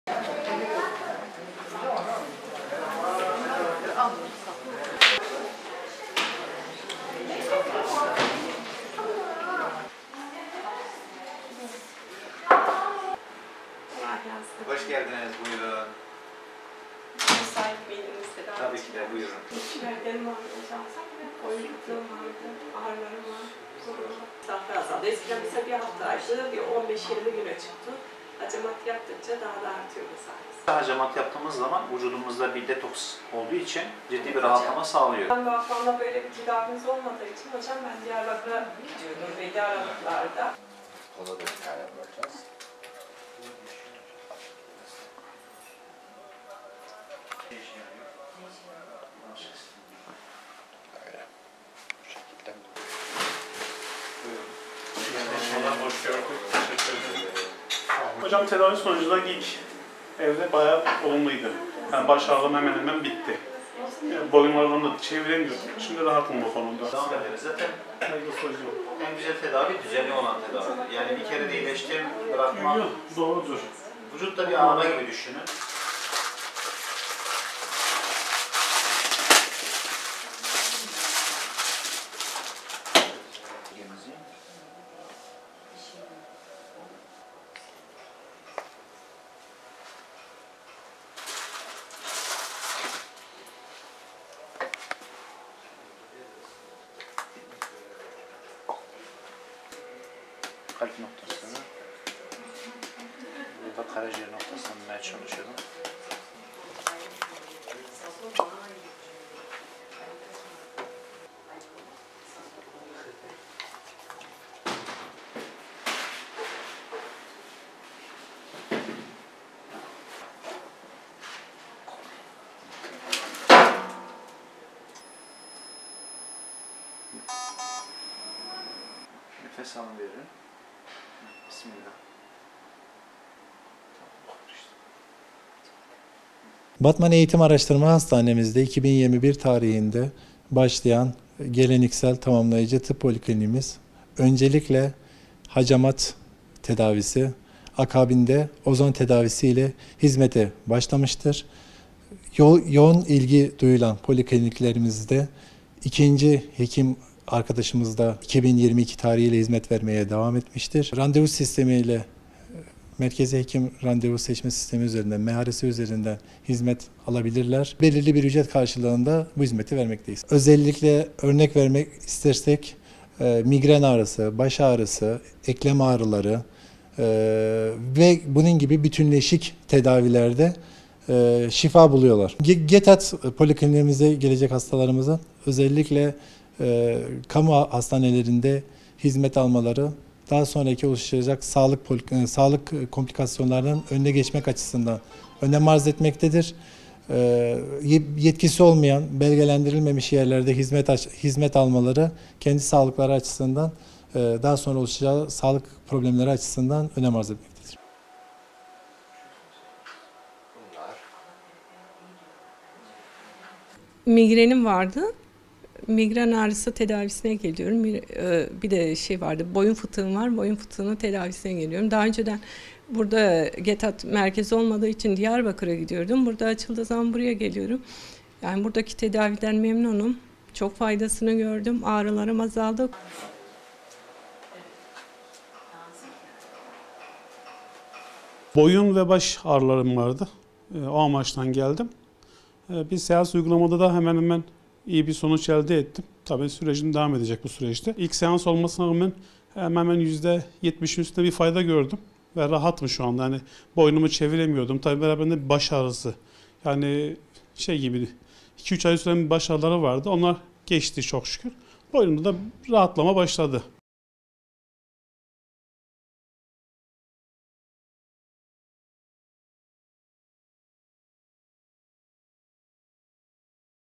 Migren, baş ve eklem ağrıları olanlar dikk
Batman Eğitim ve Araştırma Hastanesi bünyesinde 2,5 yıl önce hizmete giren Geleneksel ve Tamamlayıcı Tıp (GETAT) Kliniğine 3 bin 700 hasta başvurdu.
Batman'da 90 bin metrekare alanda, 810 yatak kapasiteyle hizmet veren hastanesi bünyesinde 2021'de GETAT Kliniği açıldı. Klinikte başta kupa terapisi (hacamat) olmak üzere ozon ve akupunktur gibi geleneksel ve tamamlayıcı tıp yöntemleri kullanılıyor. İki hekimin görev yaptığı klinikten şimdiye kadar 3 bin 700 hasta hizmet aldı.
İl Sağlık Müdürü Semih Canpolat, kliniğin hacamat ve ozon tedavisi ile hizmete başladığını, yoğun ilgi dolayısıyla klinik sayısını ikiye çıkararak akupunktur tedavisini de eklediklerini söyledi. GETAT Kliniğinden istifade etmek isteyenlerin Merkezi Hekim Randevu Sistemi'nden (MHRS) randevu alarak tedavilerini gerçekleştirebileceğini anlatan Canpolat, belirli bir ücret karşılığında hastaların bu hizmetten yararlanabileceğini aktardı. Klinikten hizmet alanların memnun bir şekilde sağlıklarına kavuştuğunu dile getiren Canpolat, "Özellikle migren, baş ve eklem ağrıları olan hastalarımız kliniğimize başvuruda bulunuyor. Kliniğe gelecek hastalarımızın özellikle kamu hastanelerinde hizmet almaları daha sonra oluşabilecek sağlık komplikasyonlarının önüne geçmek açısından önem arz etmektedir. Yetkisi olmayan, belgelendirilmemiş yerlerde hizmet almak sağlık açısından riskler barındırıyor." diye konuştu.